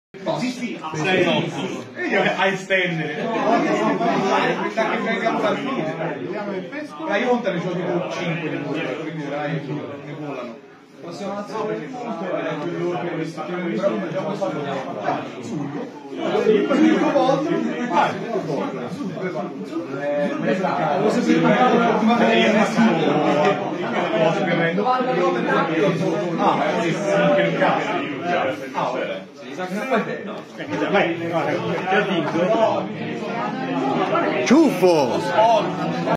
Torneo M.A.I.A.L.E. 19-12-2022 - Ciuffo Esper Imperium EDH VS Lorenzo Porciatti Selesnya Slivers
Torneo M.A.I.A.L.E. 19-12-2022
Alessio Guidi (Ciuffo) VS Lorenzo Porciatti
EDH Esper Imperium VS Selesnya Slivers
GG Good Game Empoli (FI)